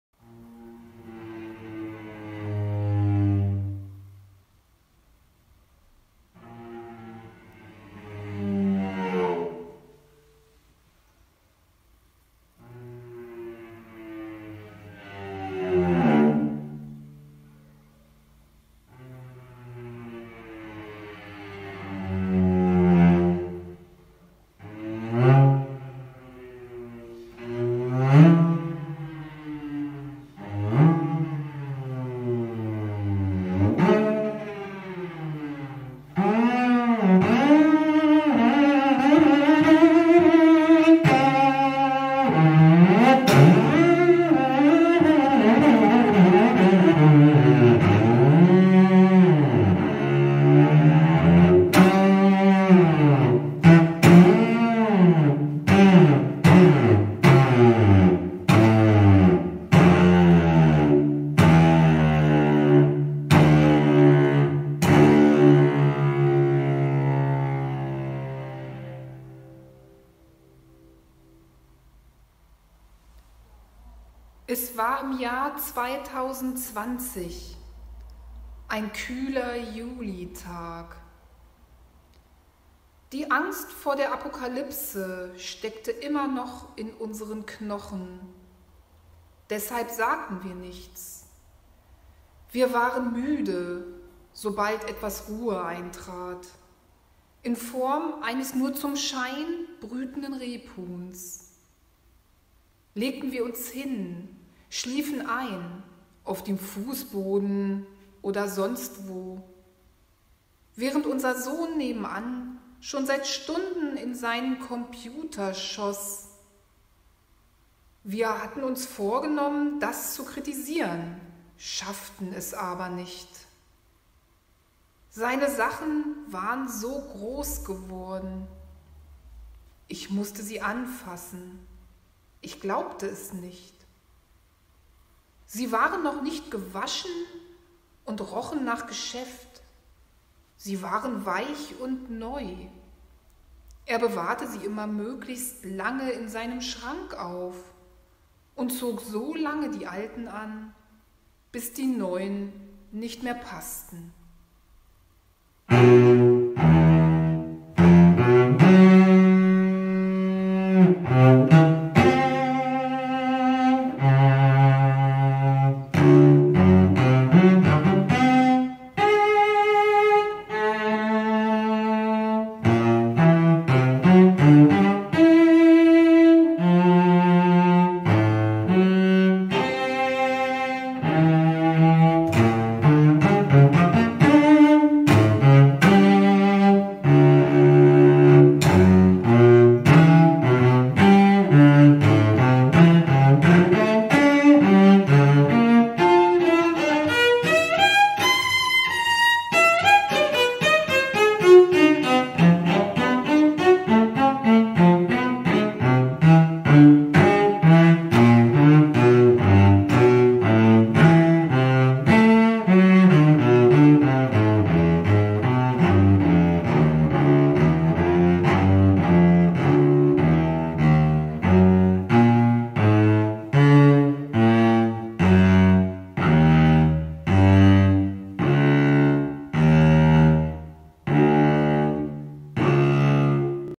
Verbindungen 04 Rebhuhn
Liveaufnahme einer Session im Frühjahr 2021

Cello: Corinna Eikmeier
Text und Bild: Christine Kappe